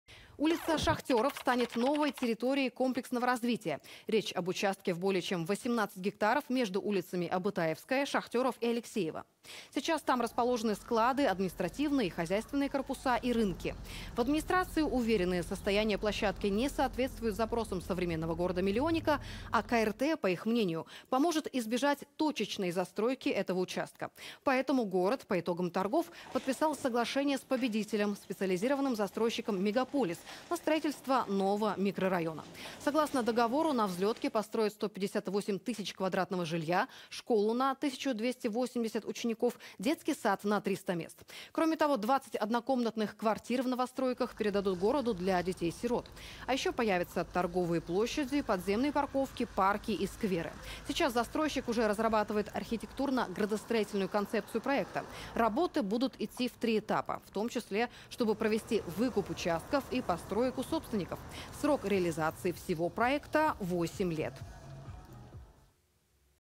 Улица Шахтеров станет новой территорией комплексного развития
Улица Шахтеров станет новой территорией комплексного развития. Речь об участке в более чем 18 гектаров между улицами Абытаевская, Шахтеров и Алексеева. Сейчас там расположены склАды, административные и хозяйственные корпуса и рынки. В администрации уверены — состояние площадки не соответствует запросам современного города-миллионника. А КРТ — по их мнению — поможет избежать точечной застройки этого участка. Поэтому город, по итогам торгов, подписал соглашение с победителем — «Специализированным застройщиком «Мегаполис», на строительство нового микрорайона. Согласно договору, на Взлетке построят 158 тысяч квадратов жилья, школу на 1280 учеников, детский сад на 300 мест. Кроме того, 20 однокомнатных квартир в новостройках передадут городу для детей-сирот. А еще — появятся торговые площади, подземные парковки, парки и скверы. Сейчас застройщик уже разрабатывает архитектурно-градостроительную концепцию проекта. Работы будут идти в 3 этапа: в том числе чтобы провести выкуп участков и построек у собственников. Срок реализации всего проекта — 8 лет.